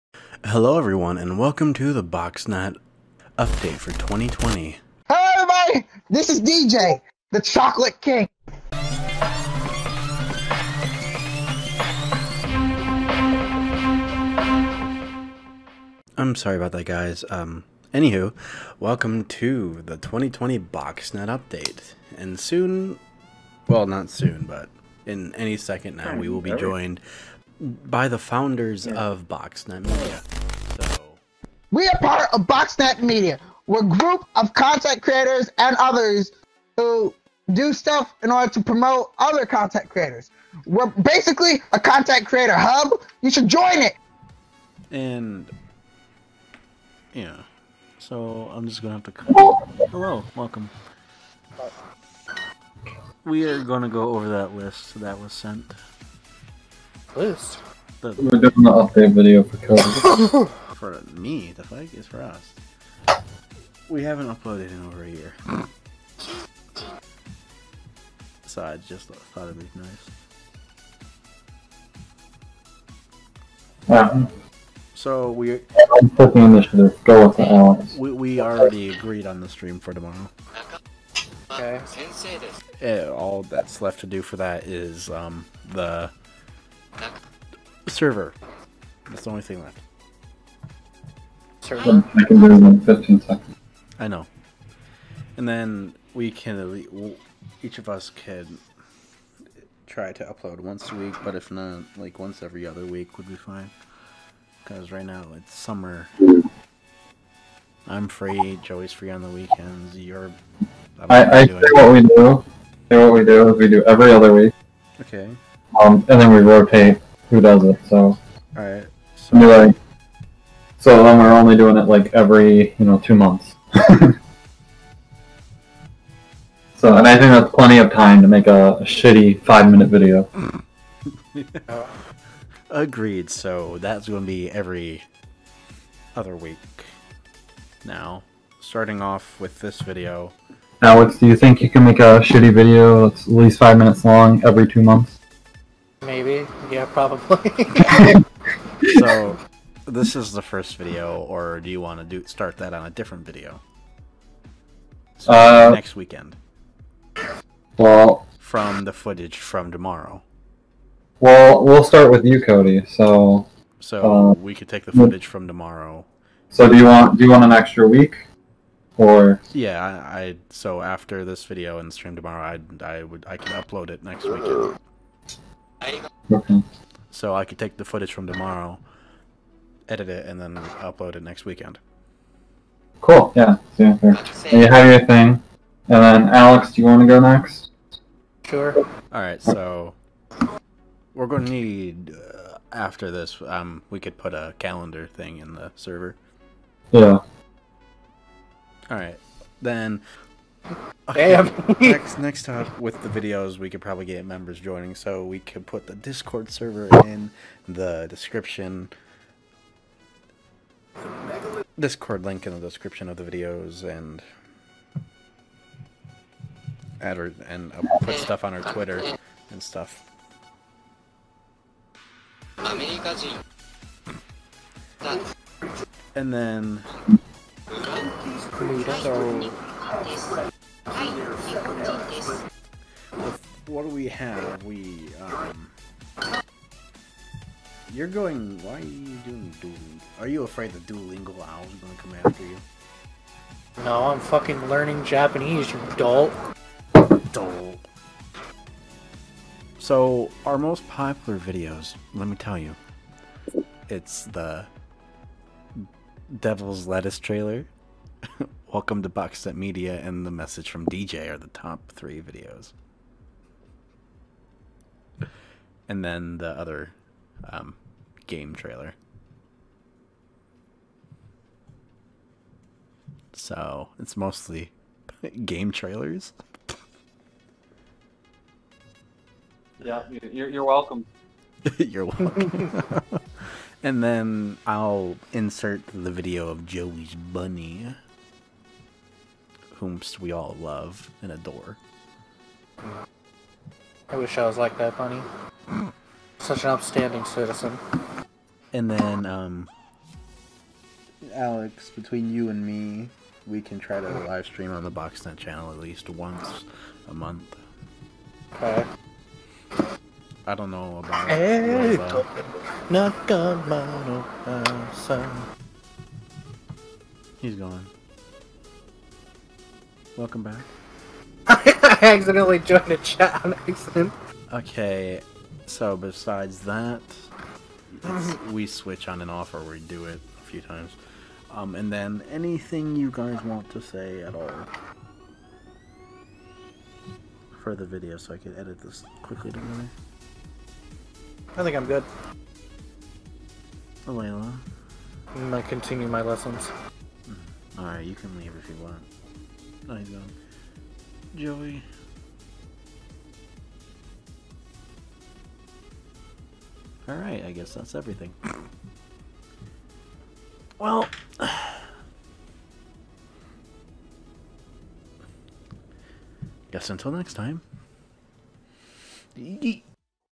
Boxnet Update 2020 and Stream Prep
Hello everyone with the current state of the world we decided to get back into the creation game. This will give you some intel of what we have in mind of what to do. Starting off with a late 2 year celebreation of the creation of Boxnet Media. We love you all and here is the discord link to our server come join us for fun times and to make new friends. Can not wait to see you there.